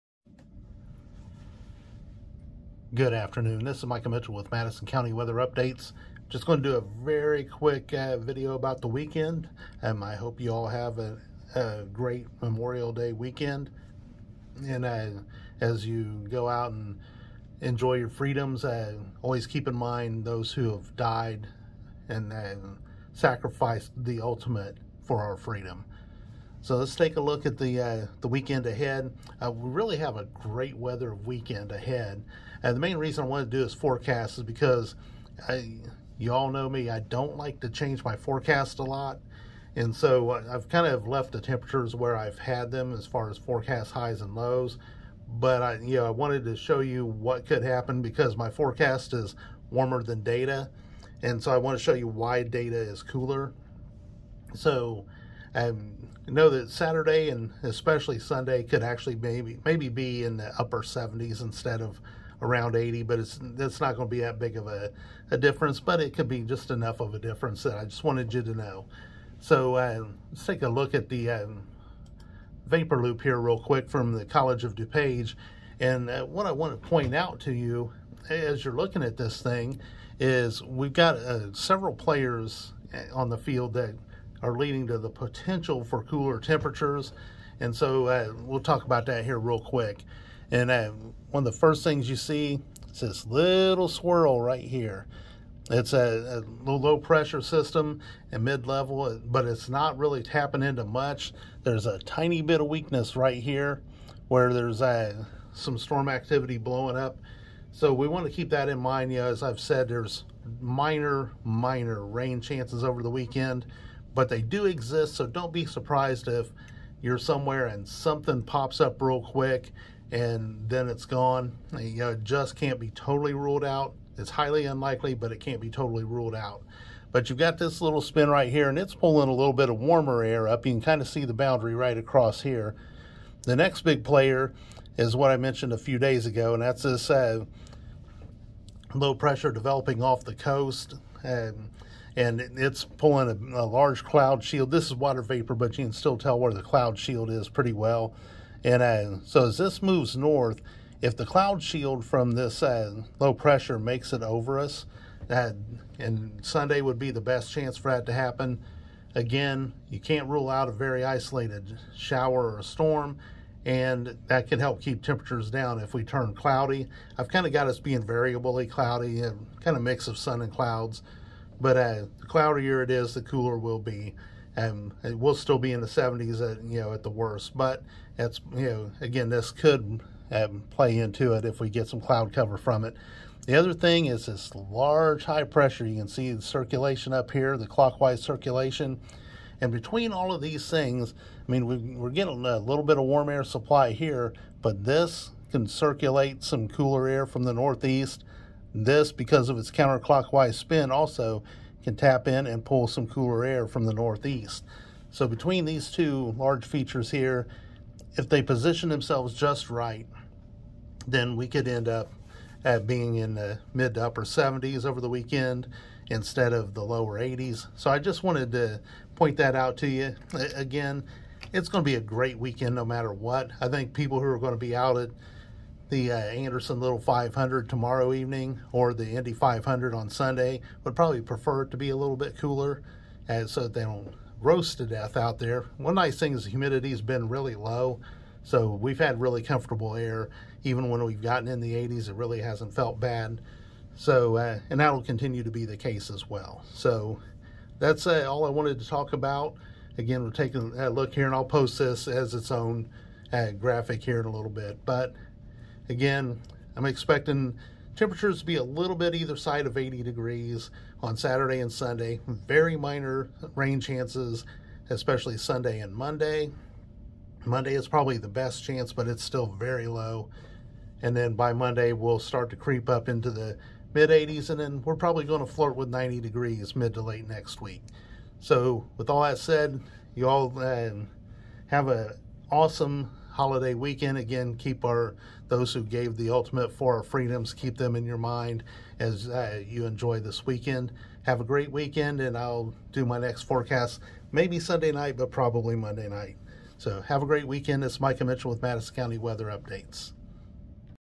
Memorial Weekend Weather 2023
Five minute video about the weekend weather and possible temperature variation, as well as tiny rain chances.